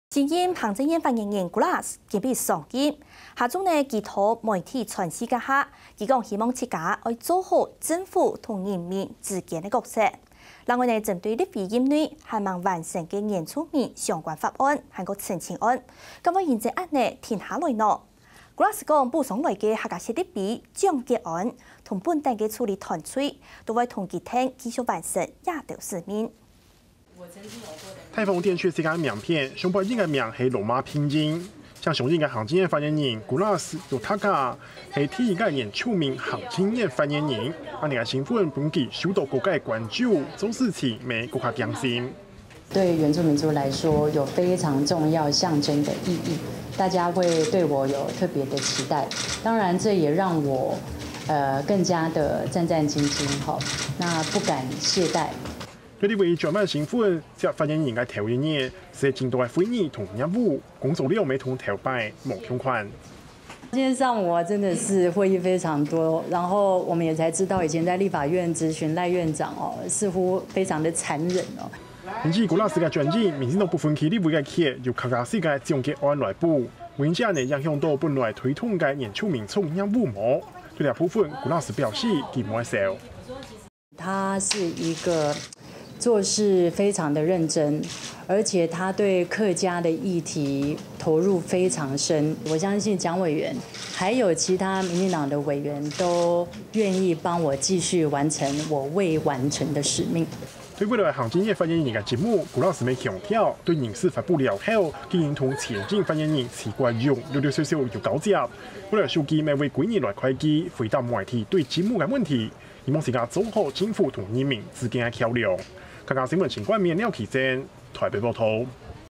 新任政院發言人 Kolas:期許做好溝通角色【客家新聞20180716】
新任行政院發言人Kolas今天走馬上任，在與媒體茶敘時，期許自己能扮演好，政府與人民之間的角色，此外對於立委任內還未完成的，原住民相關法案與陳情案，會不會因此而停滯，Kolas則表示，遞補上來的客籍立委蔣絜安，和原本的助理團隊，都願意幫助她繼續完成這些使命。